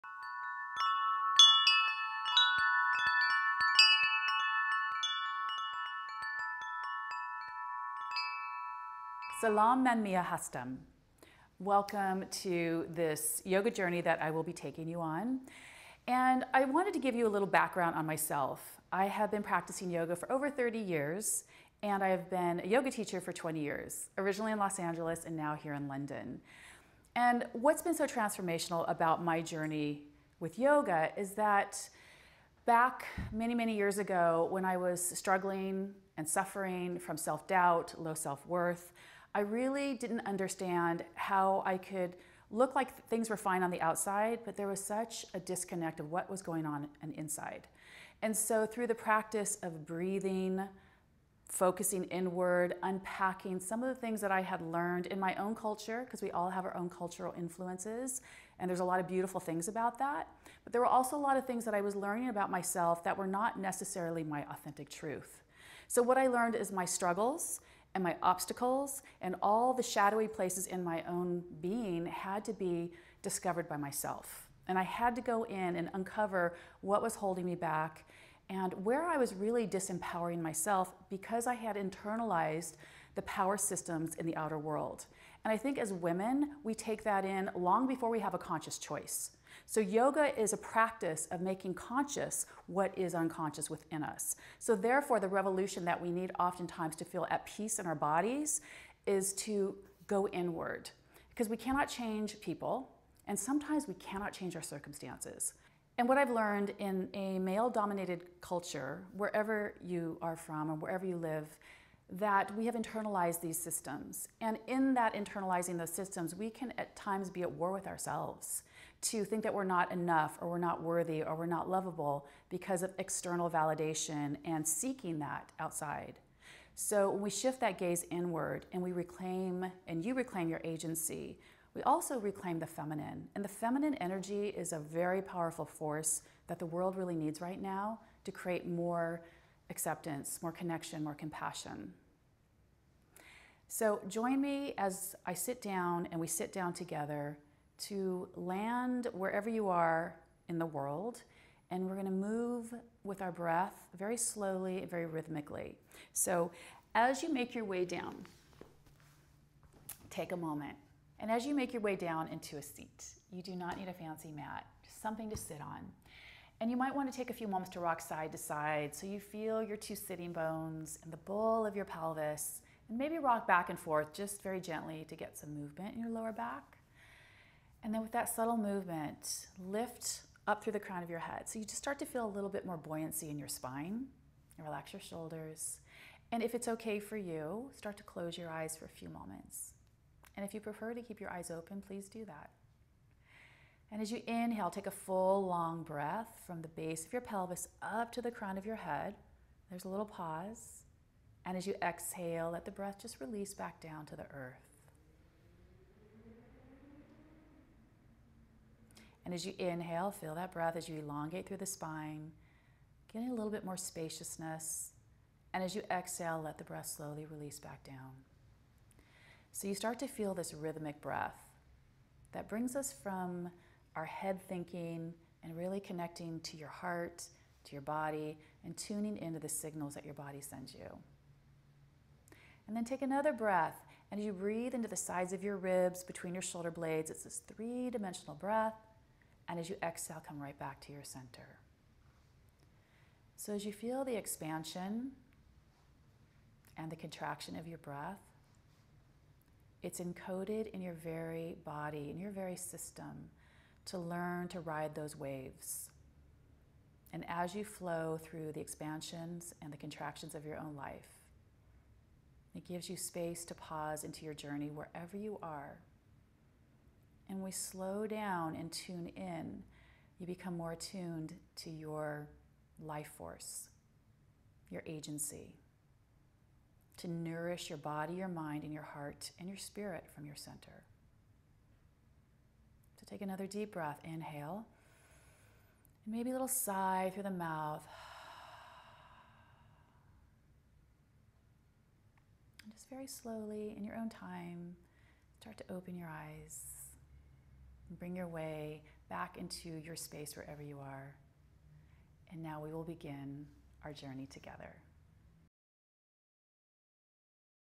Yoga with Mia Togo: Breathing
This video is part of a series of yoga exercises with Mia Togo, who has been a yoga teacher for 20 years, designed specifically for women in Iran who are either incarcerated or practice in small spaces.

Video #1: Breathing

Sit down and move with your breaths smoothly and rhythmically in order to connect to sense the signals that your body sends you.